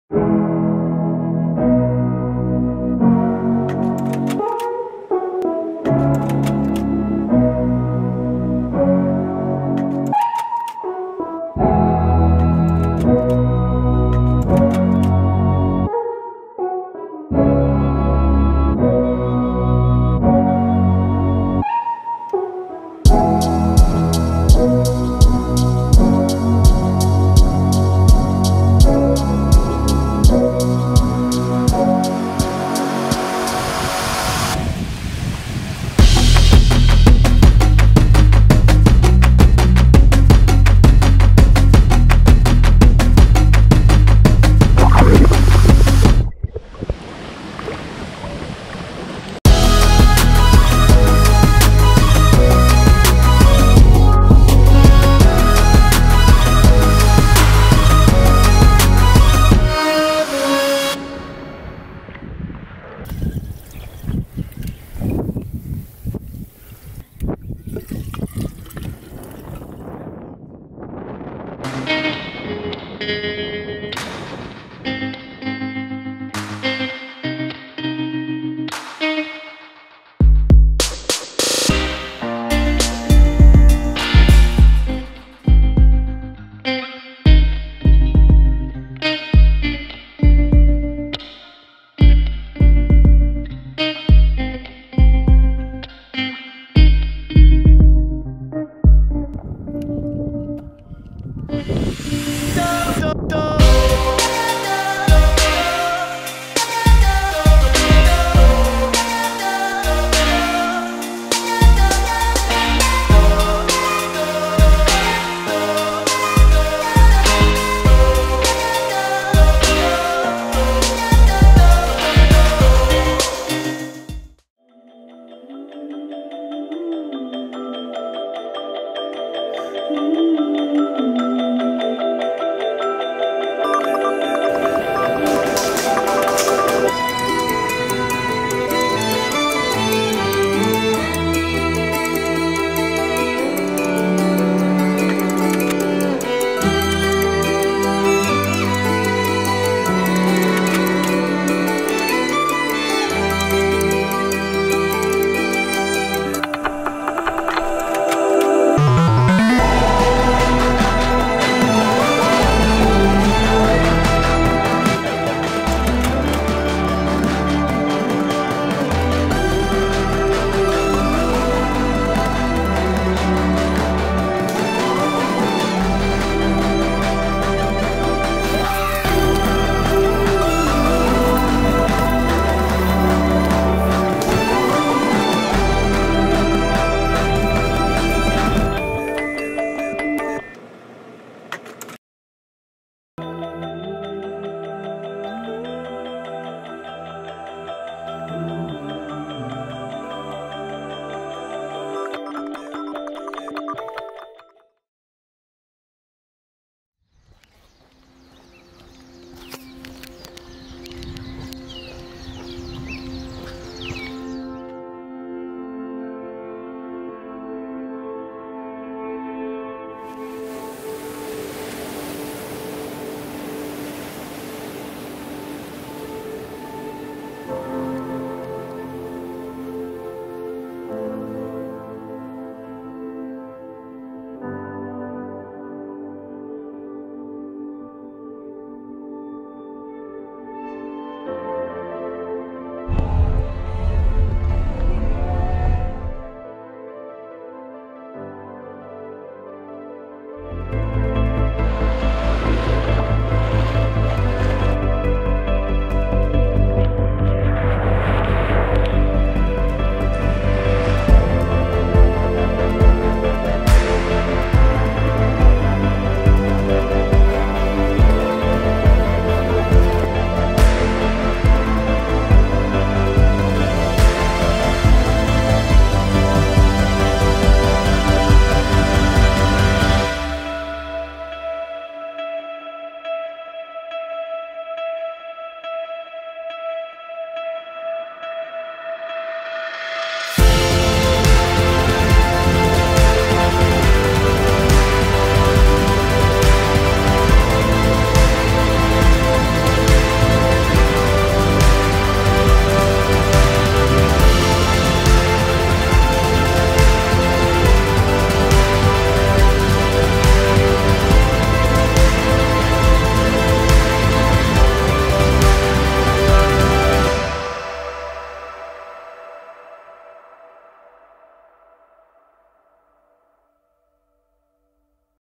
Hawaii Pt. 1 | Kauai
Part 1 of 2 of an amazing trip to Hawaii with amazing people! :)